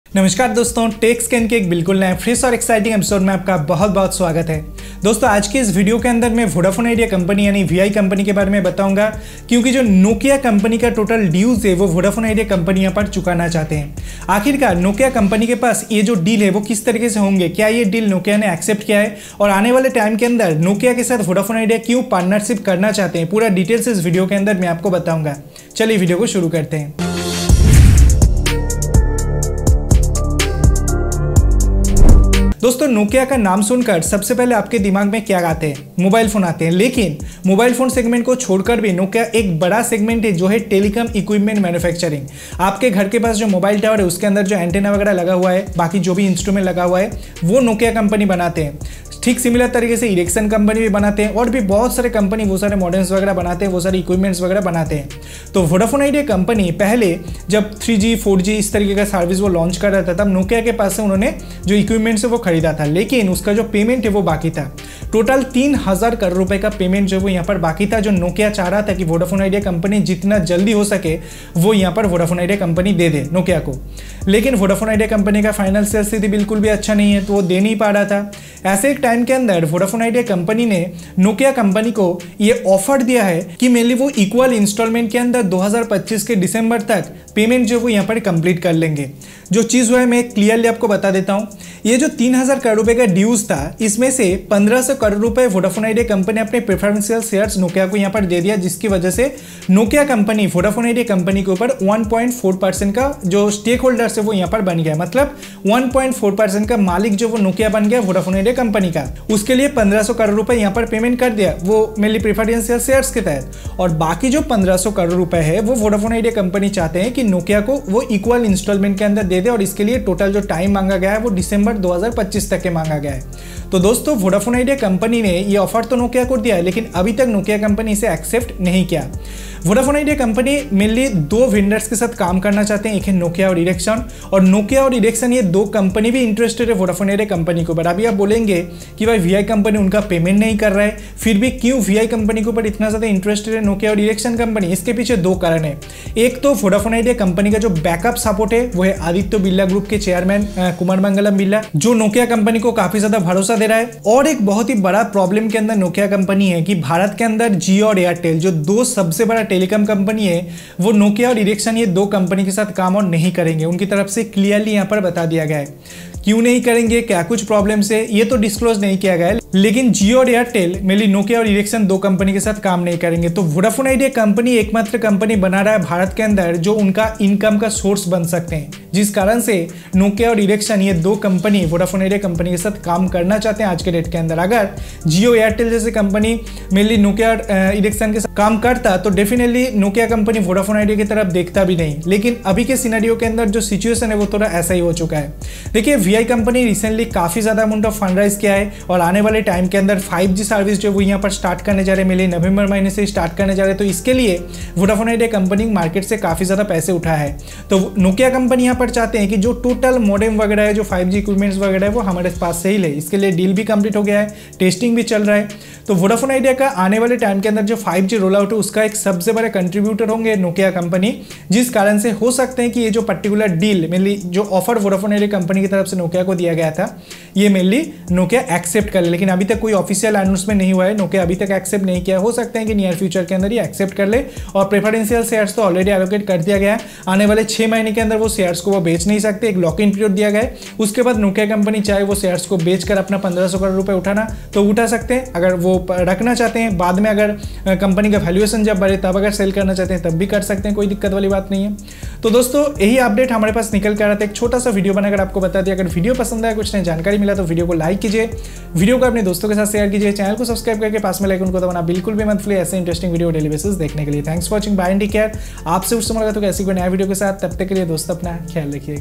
VODAFONE IDEA भारत में NOKIA का हर एक कार्ज़ा चुकाना चाहते हैं
Video Topic
Vodafone Idea Limited (VIL), the third-largest telecom operator in the country, has offered to clear the remaining Rs 1500 crore worth of dues to Nokia in equal instalments. However, Nokia has not yet accepted the offer yet. The telco wants to clear this in different instalments by December 2025. For the unaware, Vodafone Idea recently decided to allot 1027 million equity shares to Nokia via a preferential issue to clear around half of the pending dues. The remaining Rs 1500 crore can now be paid by the telco in equal instalments, subject to Nokia's approval.